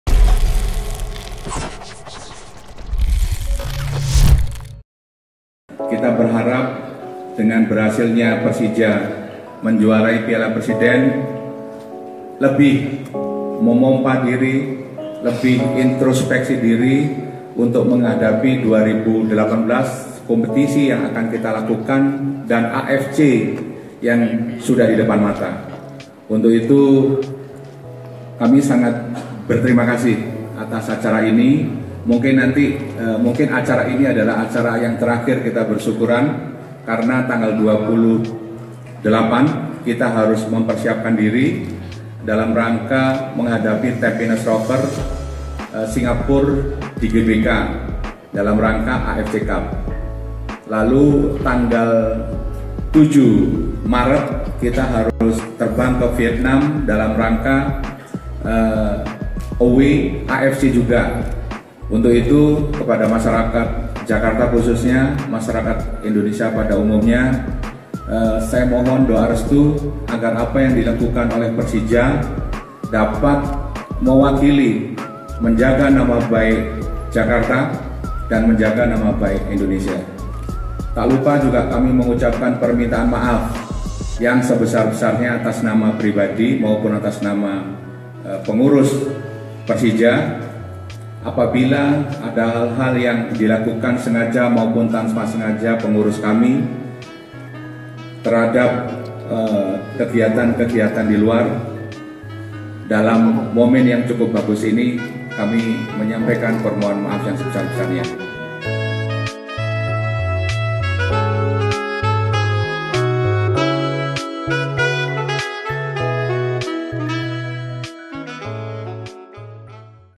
Harapan Besar Direktur Persija Jakarta Setelah Juara Piala Presiden 2018
BOLASPORT.COM - Direktur Persija Jakarta, Gede Widiade, menyampaikan harapan besar di 2018 setelah Macan Kemayoran menjadi Juara Piala Presiden 2018 di acara Syukuran yang di gelar Senin (19/2/2018).